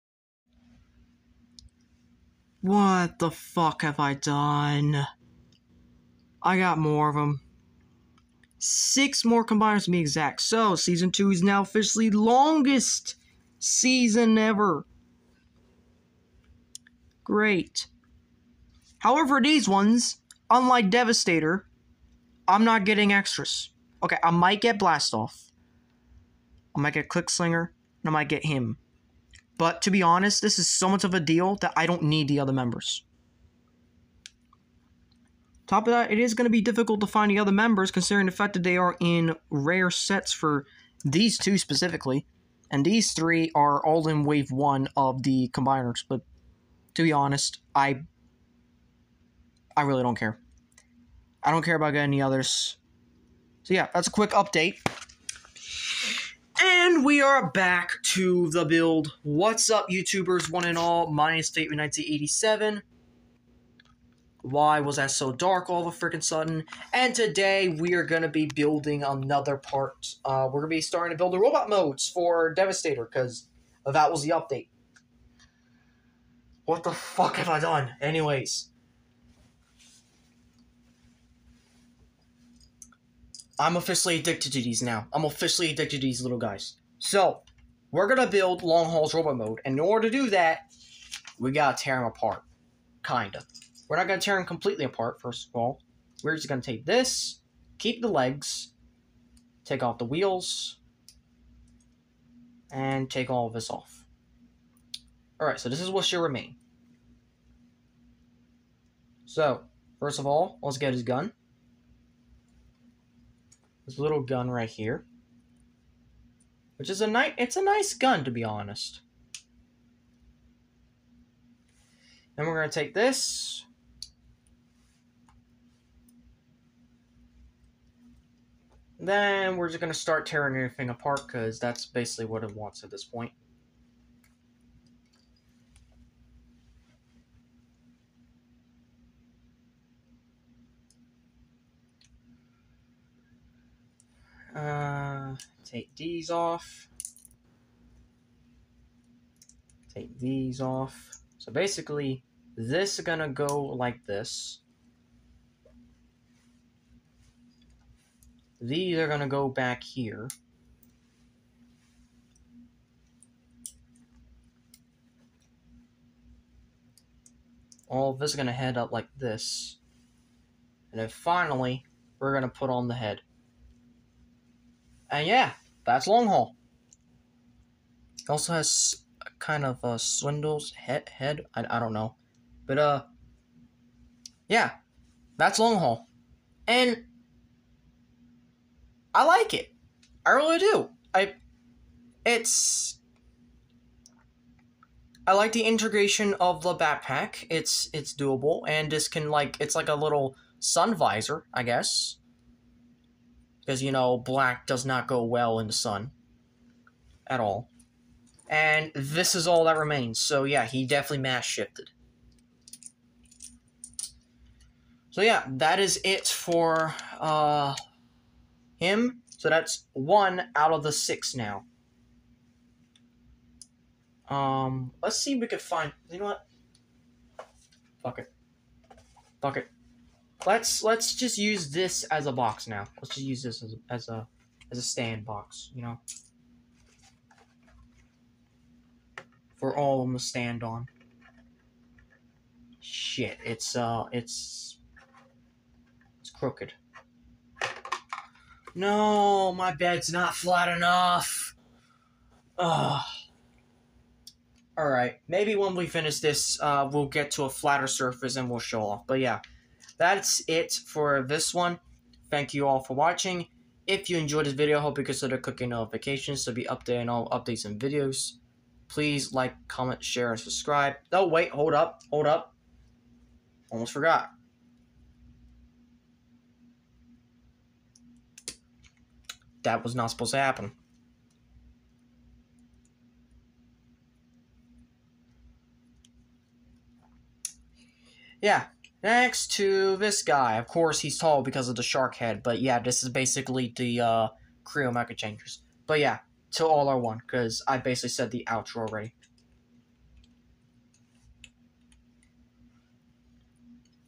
Let's Build! Episode 130: Kre-O Transformers Micro Changers Long Haul - Robot
Yes, at the very time of recording this video, I have officially instigated the second Combiner Month event. Except it's 2 months. One combiner a week.

Episode 131 will be uploaded soon.

If you enjoyed this video, consider clicking notifications bell for more updates and videos.

And as always, Til All Are One.